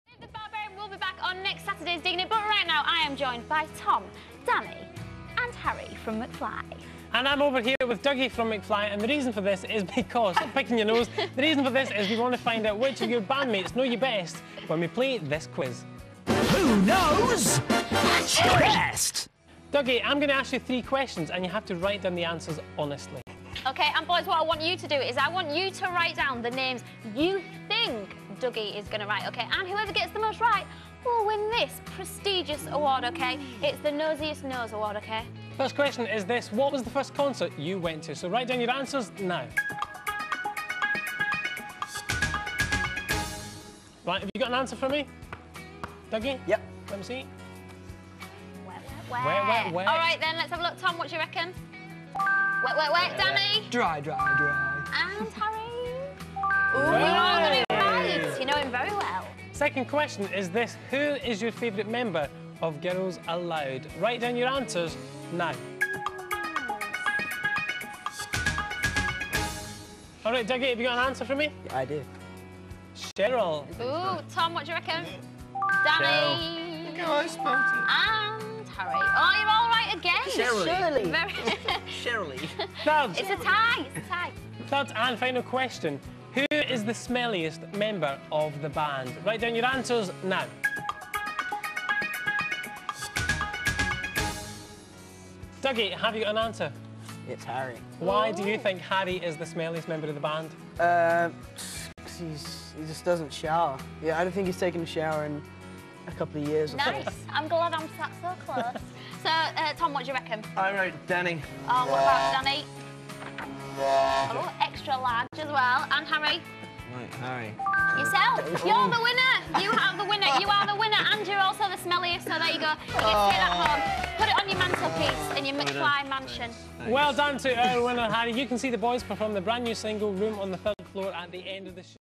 Who nose dougie best ?
Harry, Danny and Tom from McFly get to answere different questions about their basist Dougie.. Fun :)